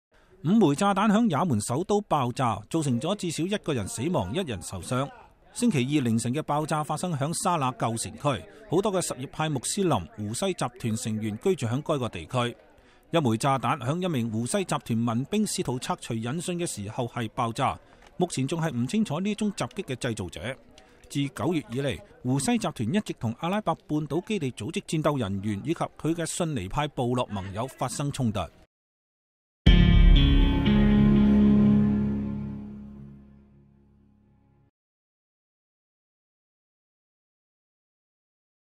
也門首都炸彈爆炸造成一死一傷
5枚炸彈在也門首都爆炸，造成至少一人死亡，一人受傷。星期二凌晨的爆炸發生在沙那舊城區，很多什葉派穆斯林胡西集團成員居住在該地區。一枚炸彈在一名胡西集團民兵試圖拆除引信時爆炸。目前還不清楚這宗襲擊的製造者。自9月以來，胡西集團一直與阿拉伯半島基地組織戰鬥人員及其遜尼派部落盟友發生衝突。